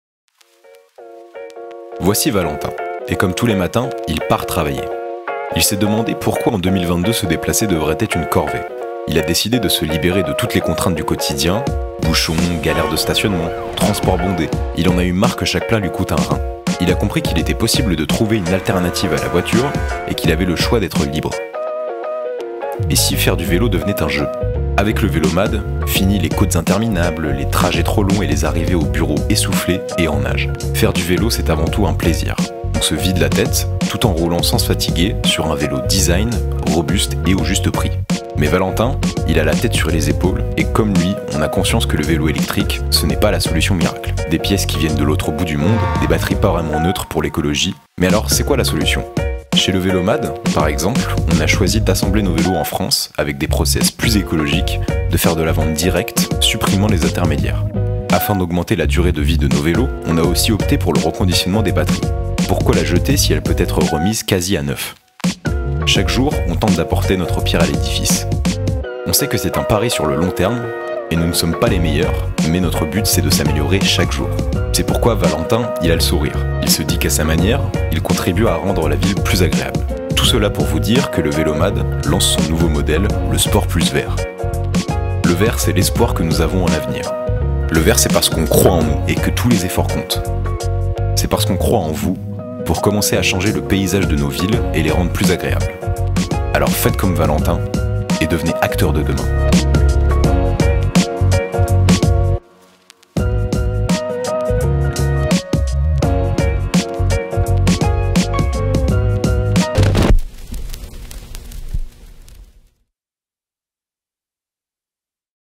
LE VÉLO MAD - LE SPORT + VERT
BORN TO RIDE.

Après avoir sorti le Sport + en février 2021, il revient dans un nouveau coloris symbolisant plus que jamais nos démarches écoresponsables.

Doté d'un moteur central à capteurs de couple, garantissant autonomie et légèreté, le Sport + reprend ce qui a fait le succès du modèle Sport, et le rend encore meilleur.